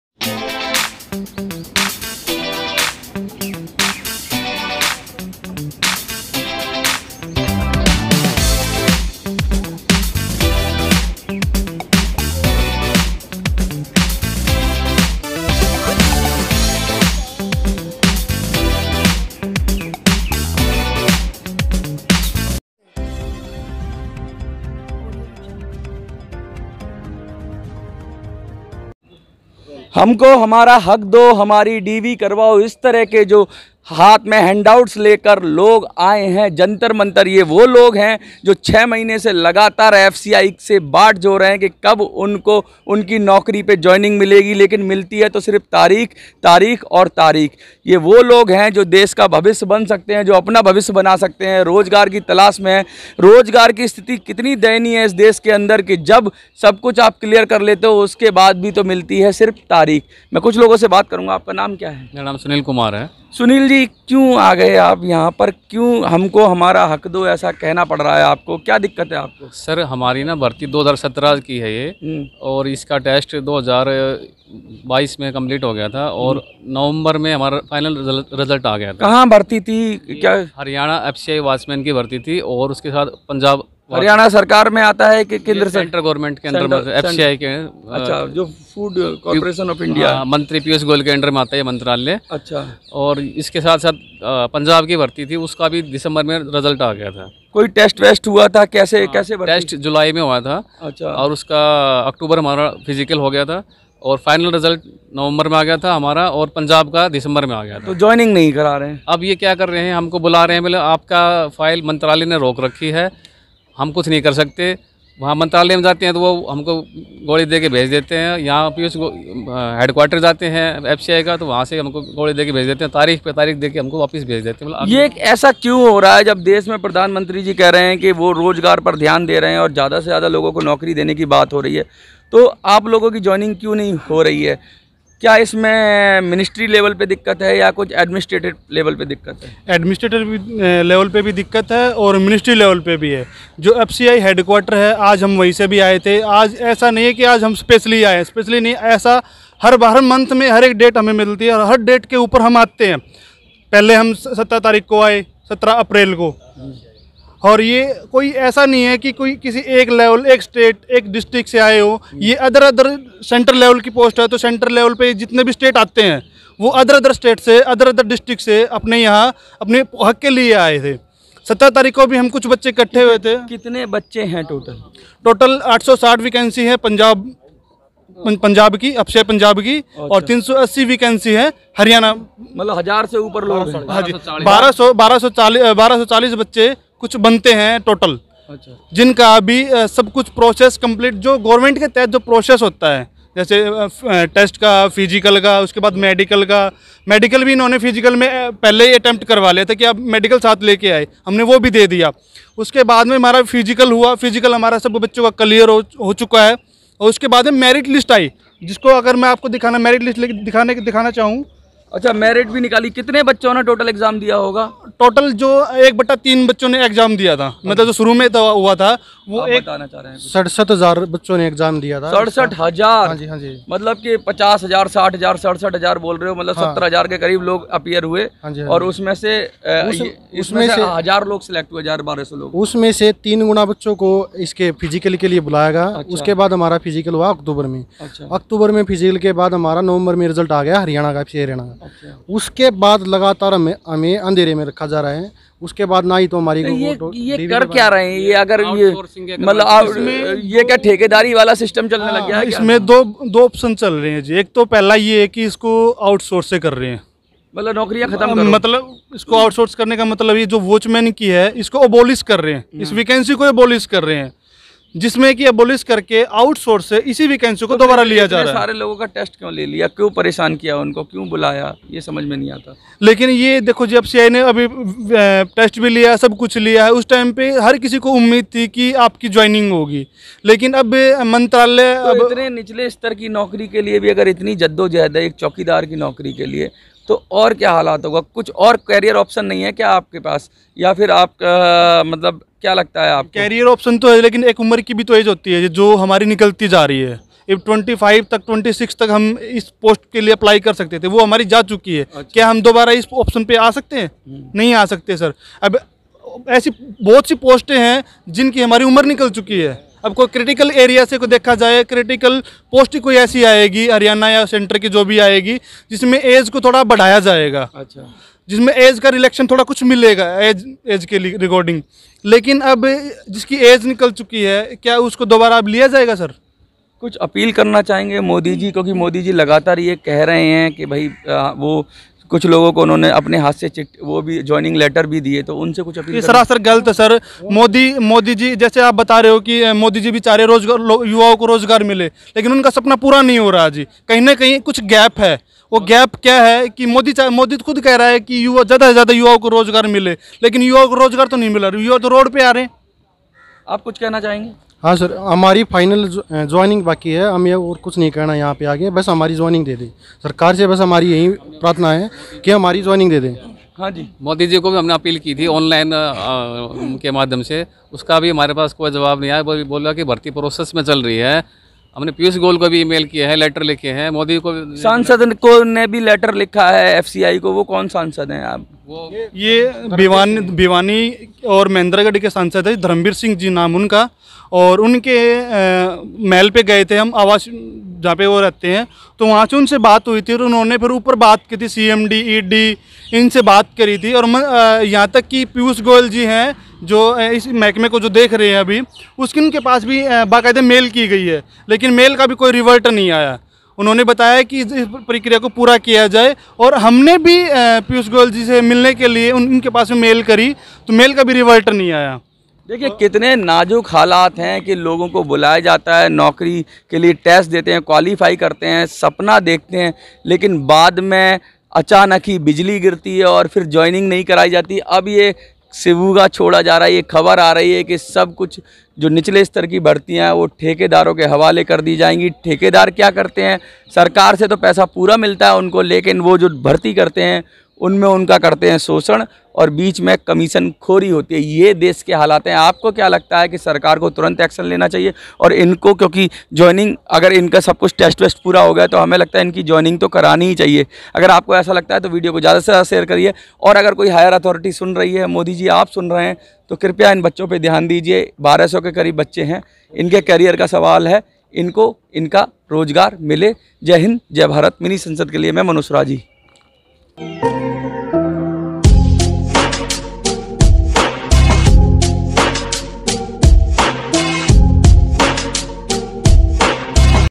ठेके पे चौकीदार तो चोरी का कौन जिम्मेदार :FCI नहीं दे रही नियुक्ति
मेरे संज्ञान में आया है कि FCI में चौकीदारों की कमी है इसके लिए भर्ती भी निकाली गई थी जिसकी सभी कार्यवाही पूरी हो चुकी है सिर्फ ज्वाइनिंग बाकी है. माननीय प्रधान मंत्री जी का सपना है कि ज्यादा से ज्यादा लोगों को रोजगार उपलब्ध कराया जाए, लेकिन इस भर्ती के सभी चयनित उम्मीदवारों ज्वाइन न कराकर इन लोगों को रोजगार से दूर किया जा रहा है. मेरे संज्ञान में यह भी आया है कि इस भर्ती को रोककर अस्थाई सदस्य लेना चाह रहे है. जो कि इनके साथ गलत होने जा रहा है.

मेरा आपसे निवेदन है कि इस भर्ती के सभी सदस्यों को ज्वाइन कराया जाए. इसके सभी दस्तावेज इस पत्र के साथ सलंग्न है.